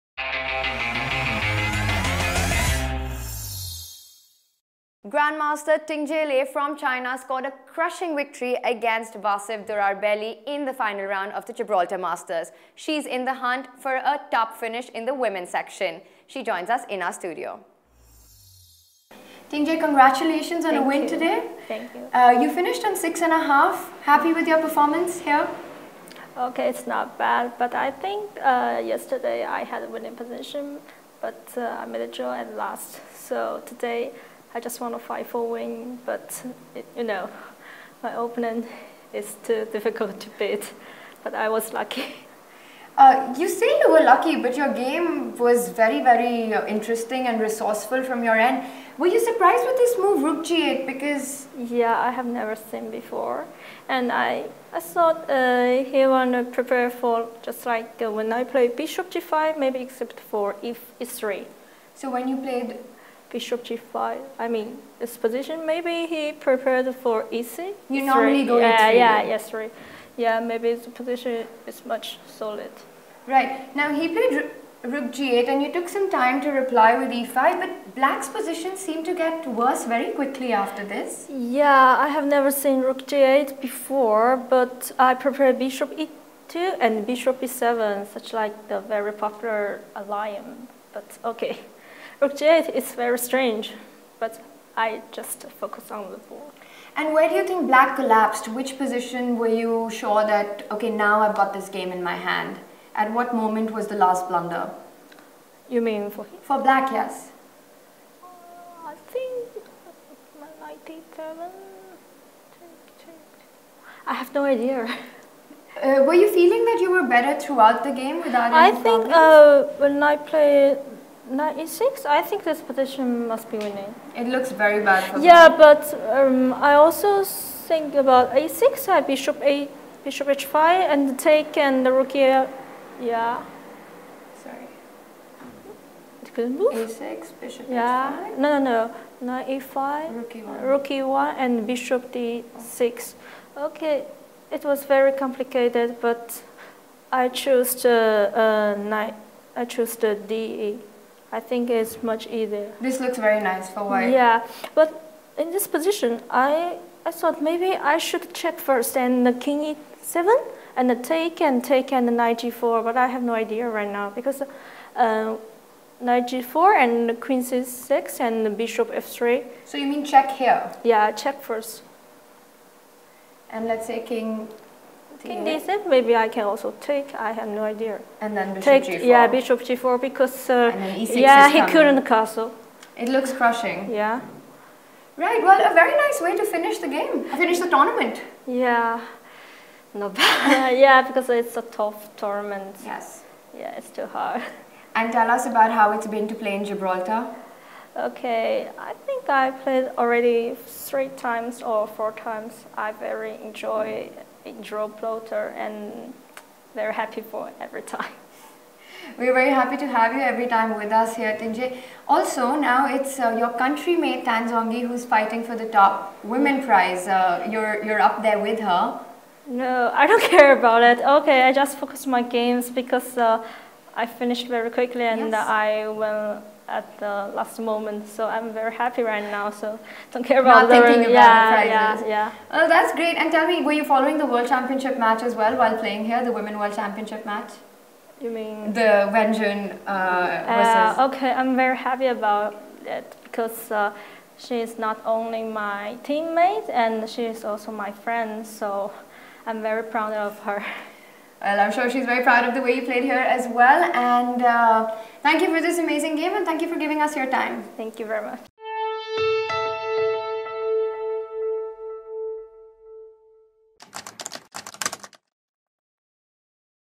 Final Round GibChess interview with Lei Tingjie
Chinese grandmaster Lei Tingjie analyses her game in the final round of GibChess